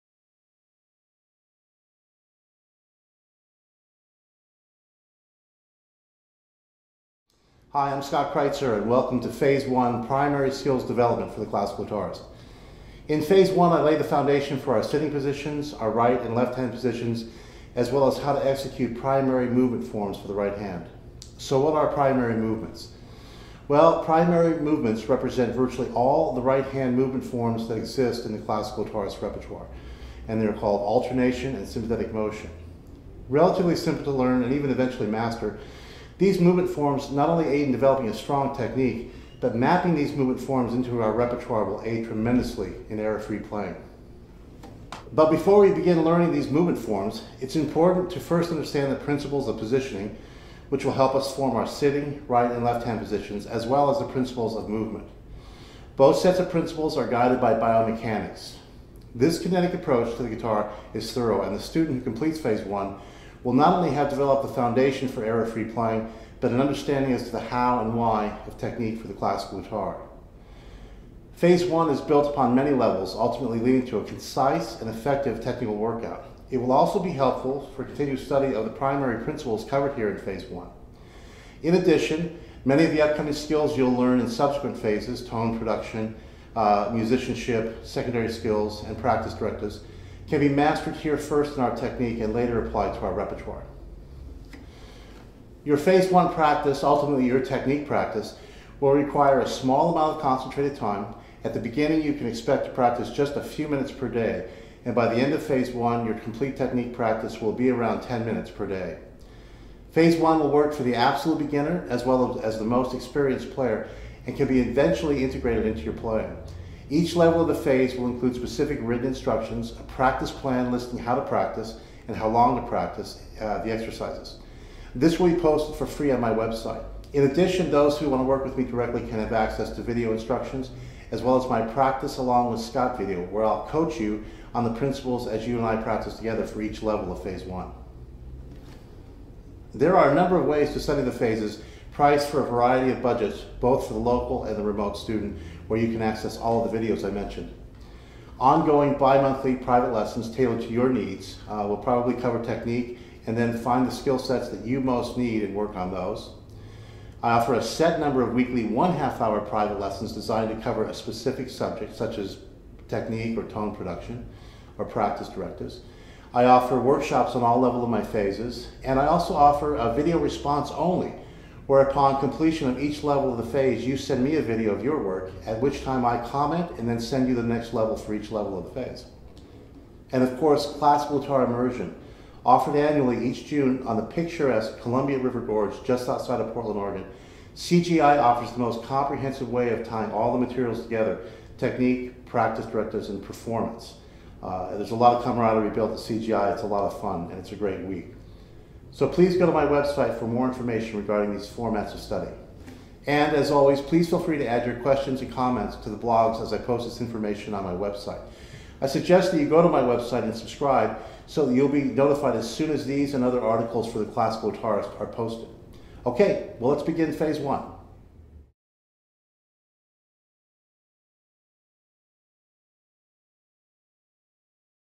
Introduction to Primary Skills Development for the Classical Guitarist by Scott Kritzer
Learn about Scott Kritzer's  Phase I, Primary Skills for the Classical Guitarist.